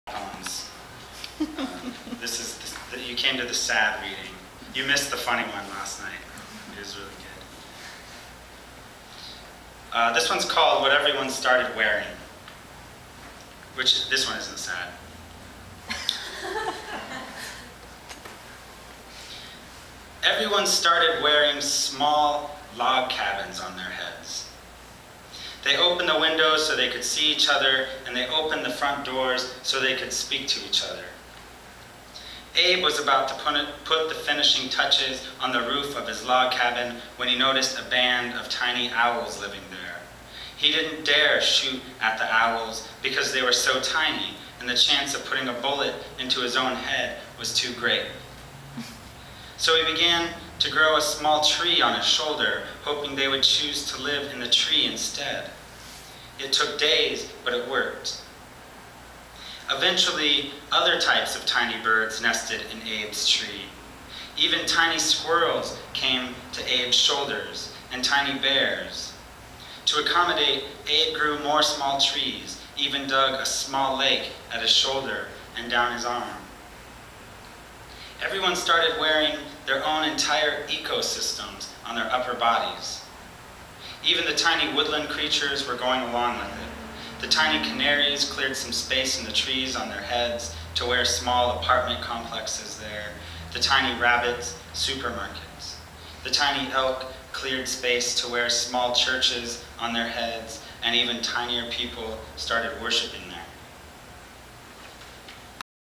Zachary Schomburg at the New york Public Library
Zachary Schomburg reading from The Man Suit at the NYPL.